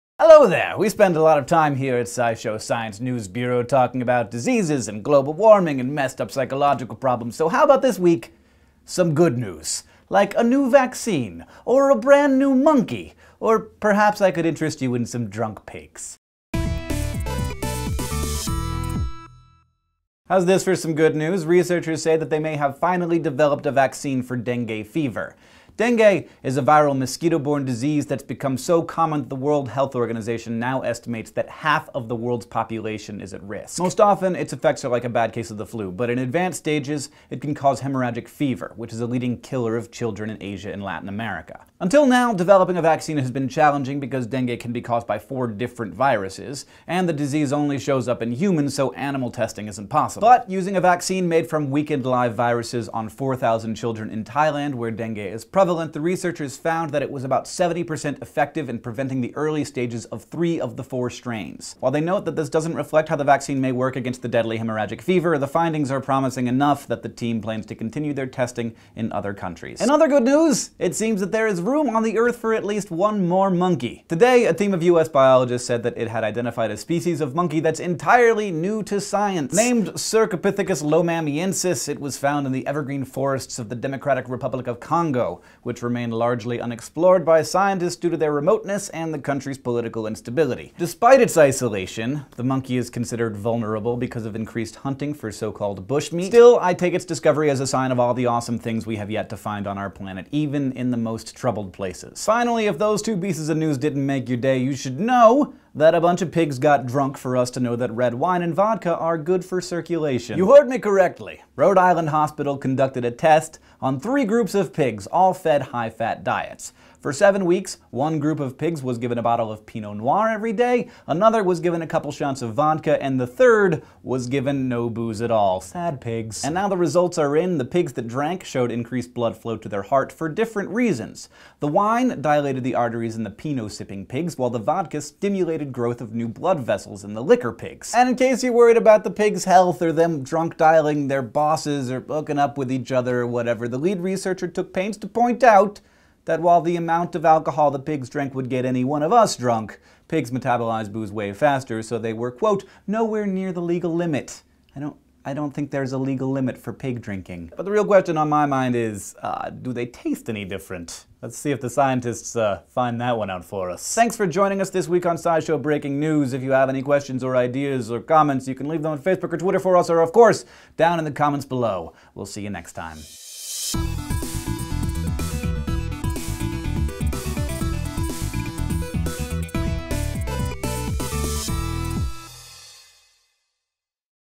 Good News, & Drinking Pigs
The SciShow Science News Bureau brings us some GOOD news this week - Hank tells us about a newly developed vaccine for dengue fever, a newly discovered monkey species in the Democratic Republic of the Congo, and some happy pigs drinking booze for science.